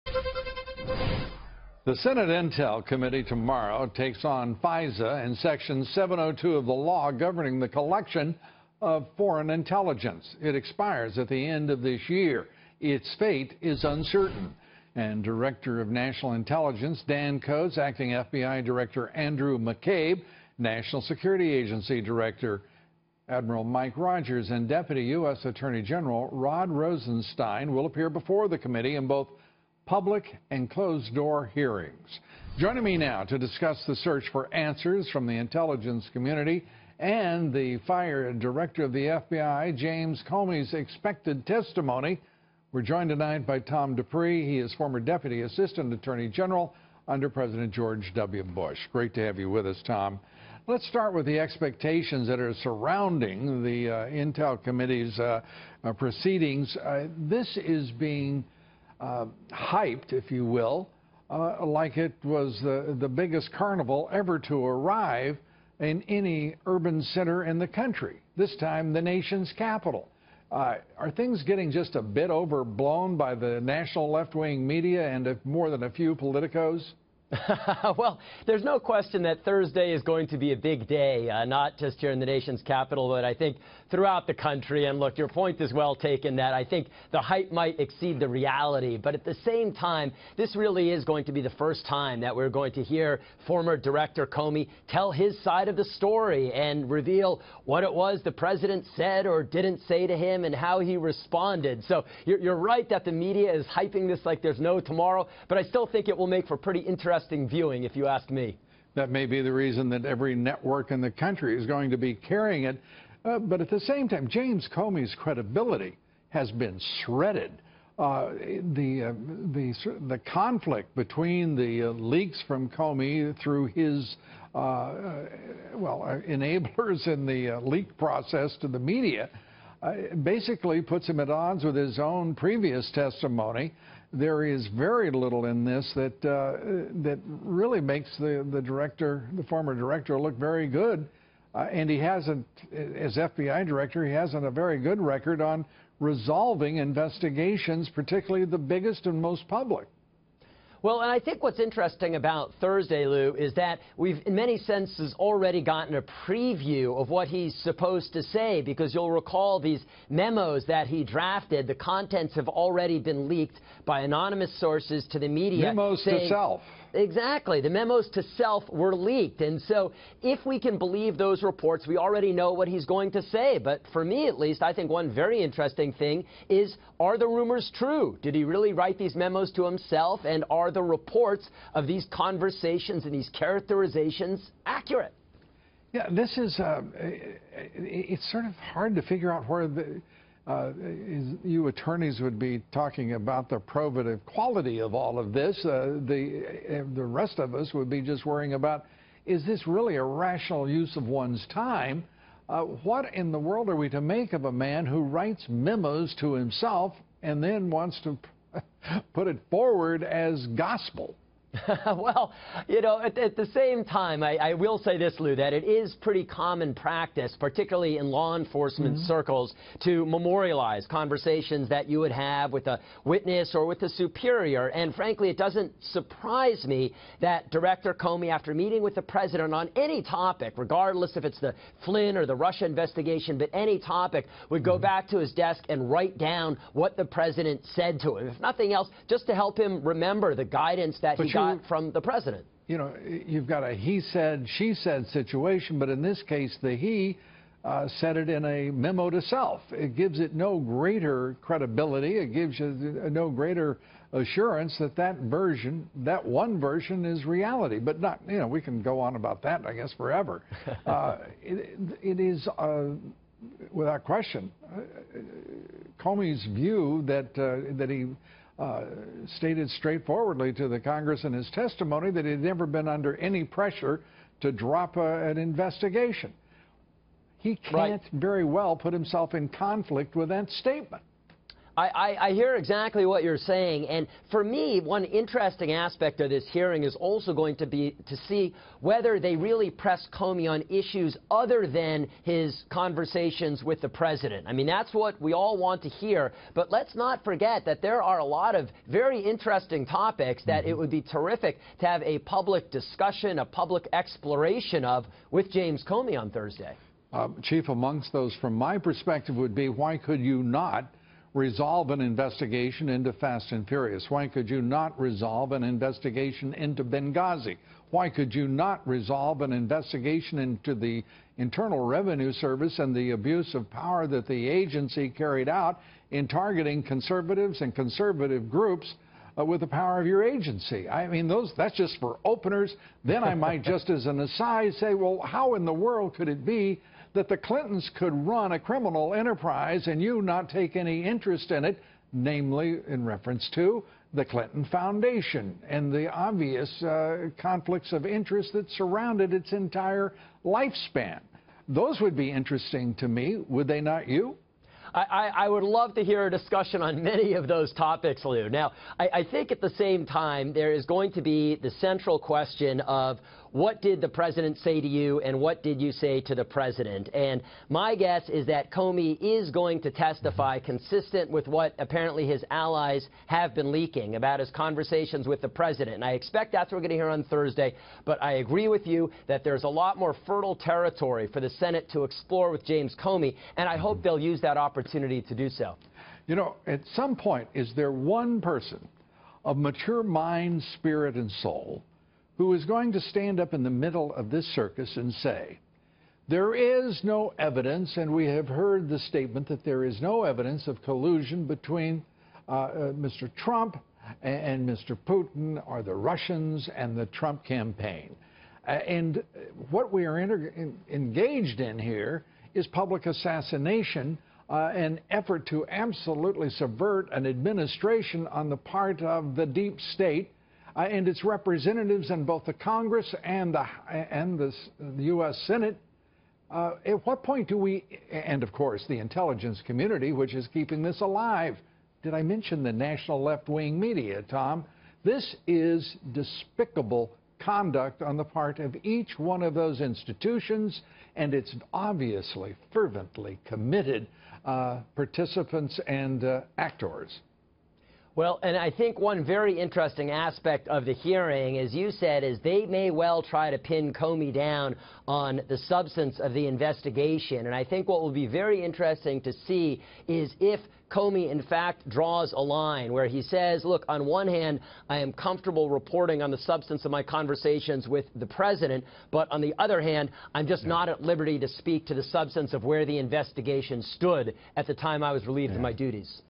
What to expect from Comey’s testimony before the Senate
Former Bush Deputy Assistant Attorney General Tom Dupree on former FBI Director James Comey’s testimony before the Senate Intelligence Committee.